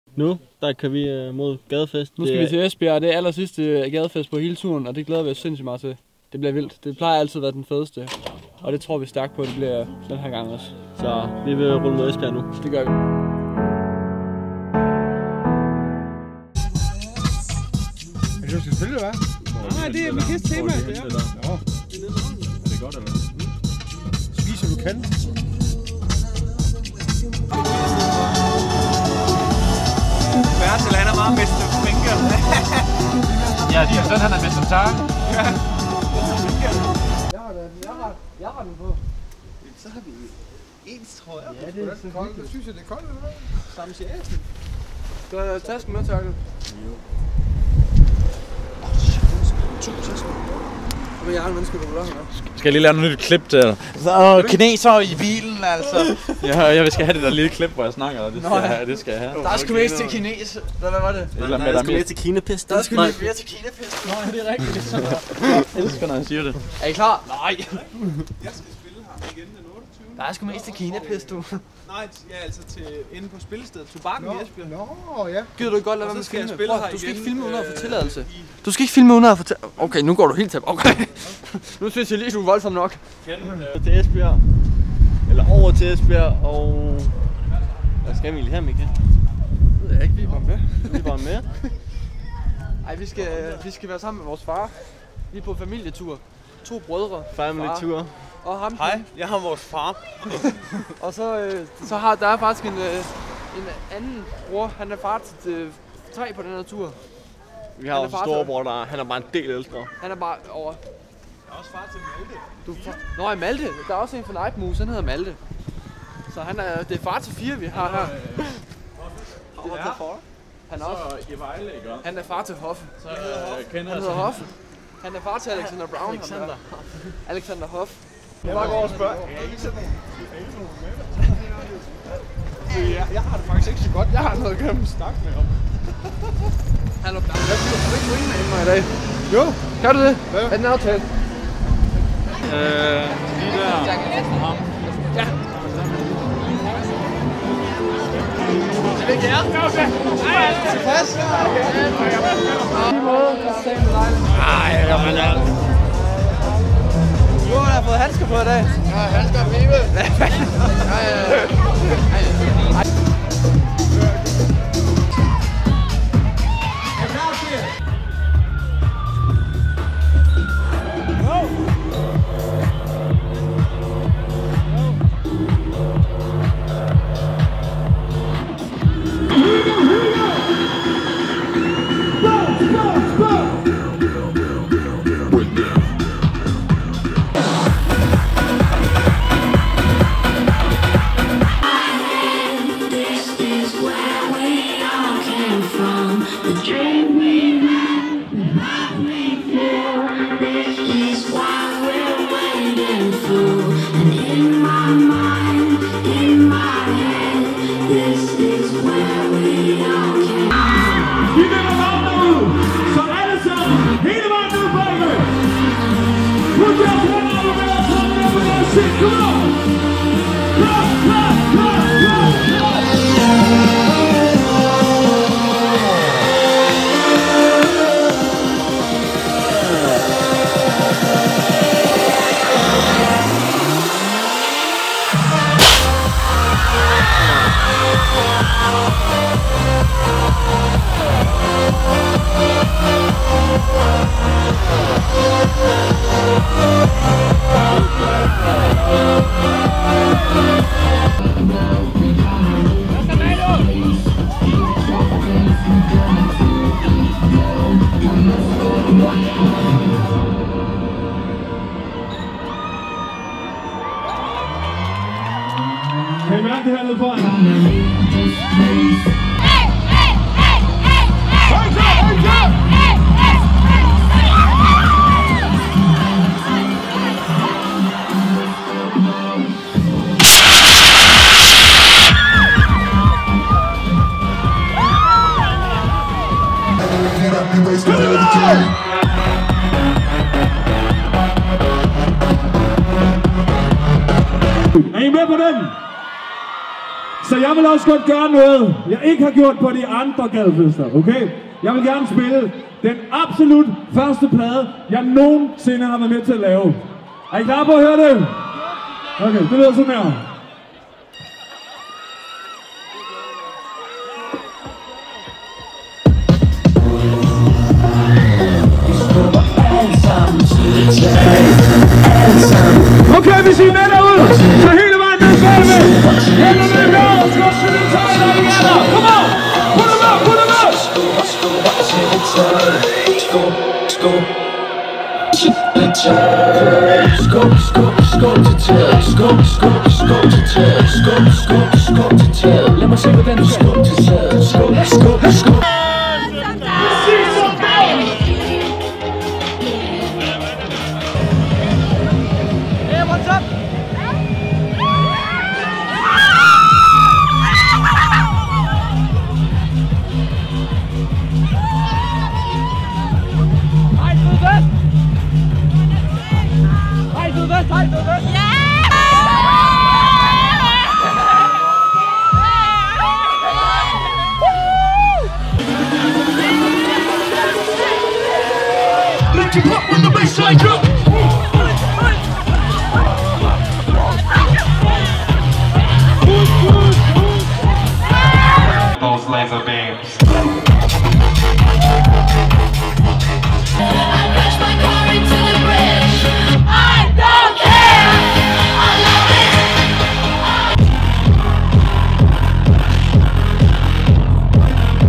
Vildeste Gadefest! Kom med på den sidste i Esbjerg
Vi har været rundt til næsten alle gadefester rundt i danmark og filme vlogs derfra. Det her er den sidste video fra gadefest for og fedeste gadefest for dette år.